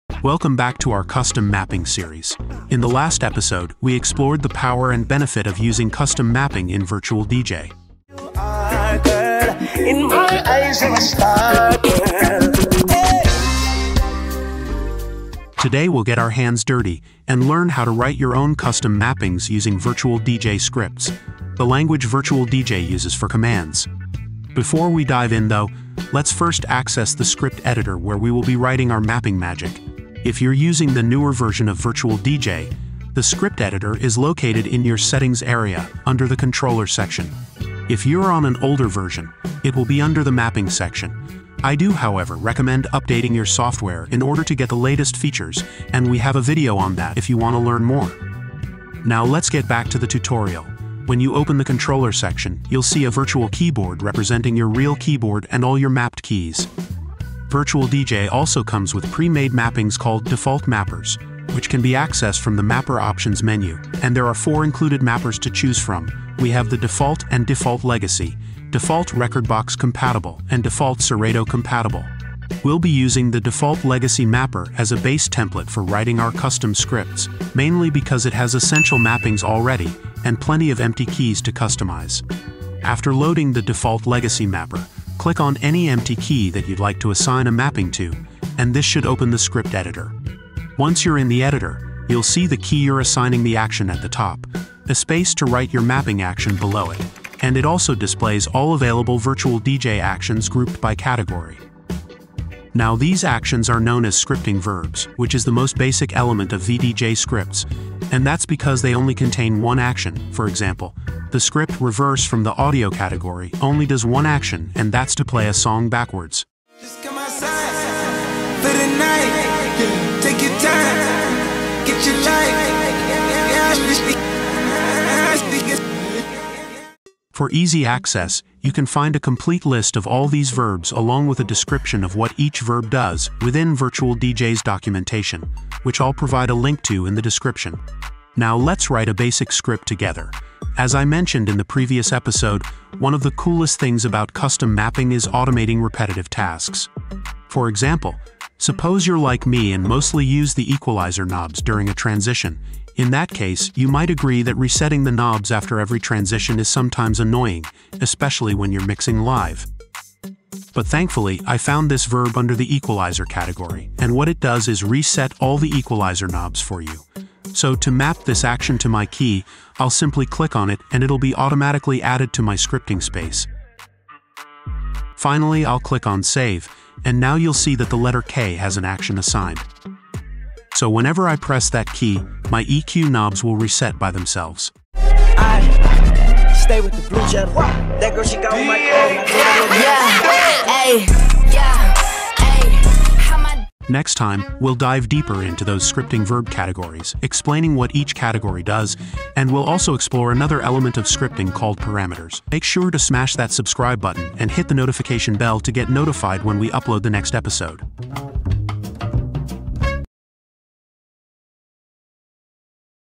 CUSTOM MAPPING EP 2 - Writing Your First Custom Mapping | Virtual DJ 2024 tutorials
CHAPTERS
0:00 Intro
0:26 VDJ Script Editor
1:44 Scripting Verbs
2:19 Writing Basic Scripts

Take your DJ skills to the next level with the power of VDJscripting!

In this episode of our custom mapping series, we dive into the world of VDJscripts, the language that unlocks automation magic in VirtualDJ 8.  Whether you're a beginner DJ or a seasoned pro, VDJscripting can transform your workflow by automating repetitive tasks and giving you more creative control.

* How to access the script editor in VirtualDJ
* What scripting verbs are and how they work
* Writing your first script to automatically reset your EQ knobs (say goodbye to those mid-mix knob fumbles!)
* An introduction to script categories and their functions

IMPORTANT LINKS